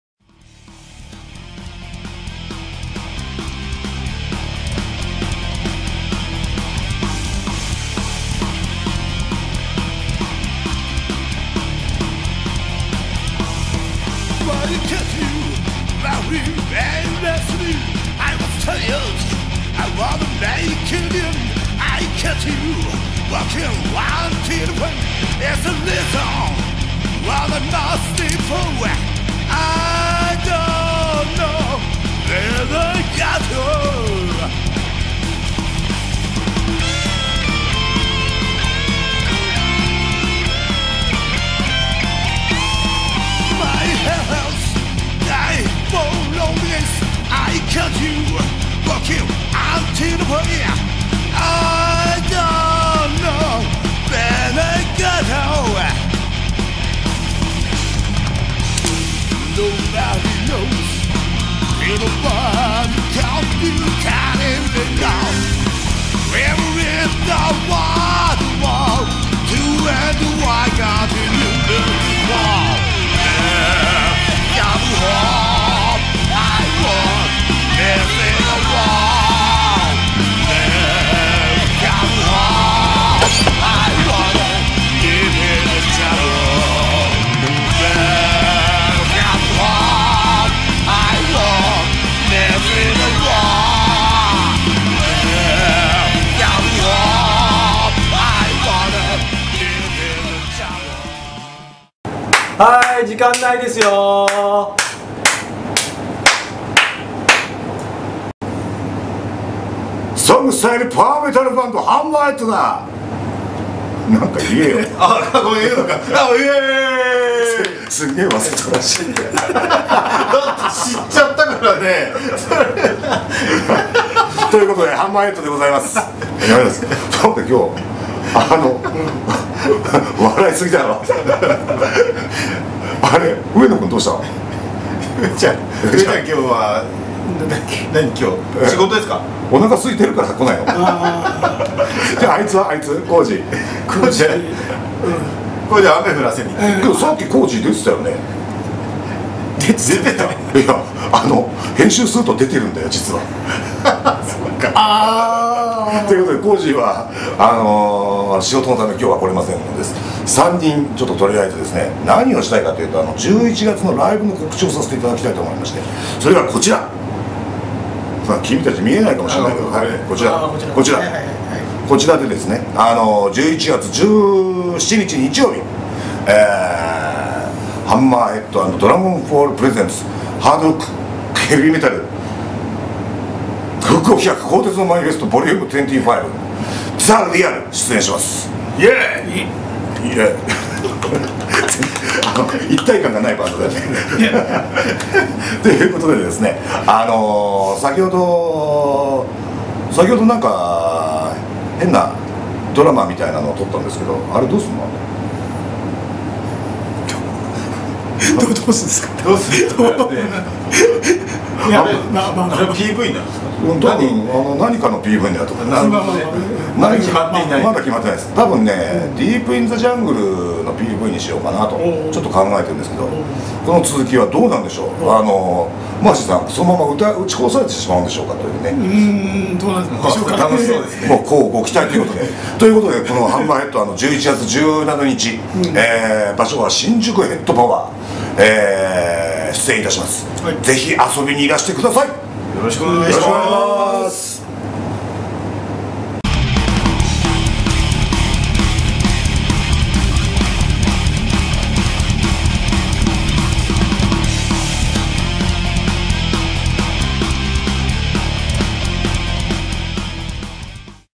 HAMMERHEAD ライブ告知（2013.11.17.）
HAMMERHEADの11/17　HR/HM復興企画「鋼鉄のManifesto Vol.25」～The REAL～ライブ告知です。